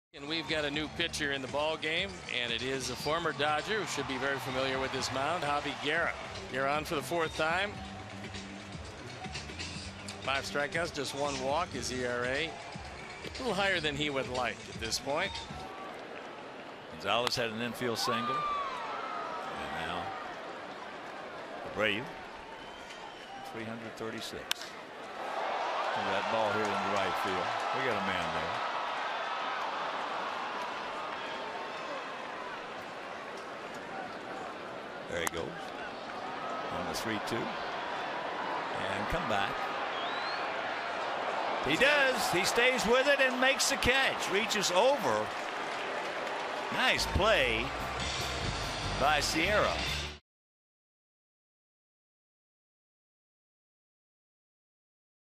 CWS@LAD: Guerra pitches an inning of scoreless relief
6/2/14: In one inning of relief, Javy Guerra allows no hits and no runs for the White Sox